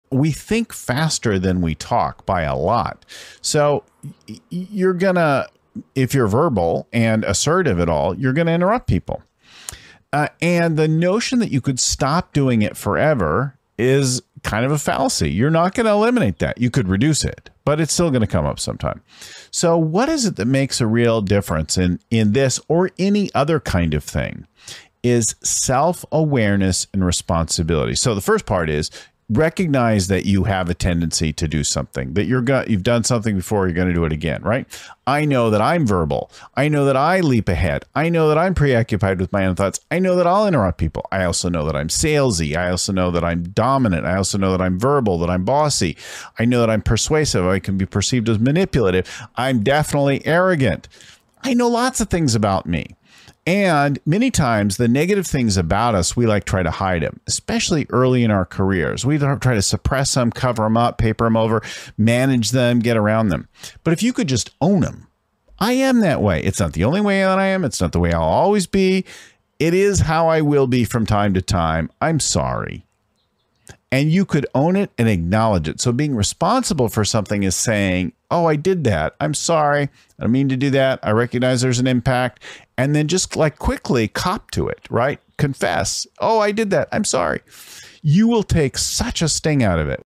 Why You Need To Own Your Habits
The key to overcoming bad habits is self-awareness and responsibility.

It can be a bit of a fallacy to think we can completely eliminate all of our bad habits. However, if you acknowledge those habits and call yourself out when you fall into those habits, your relationships will significantly benefit from that approach.

What are some ways you can own your bad habits?